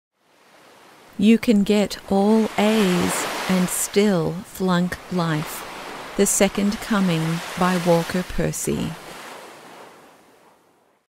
life ("The Second Coming" - Walker Percy) (Think About It)
"The Second Coming" (Walker Percy)

"Think About It: Quotations for Contemplation and Reflection" is a series of quotations from books I have read. I hope you find these to be inspiring and helpful in your life.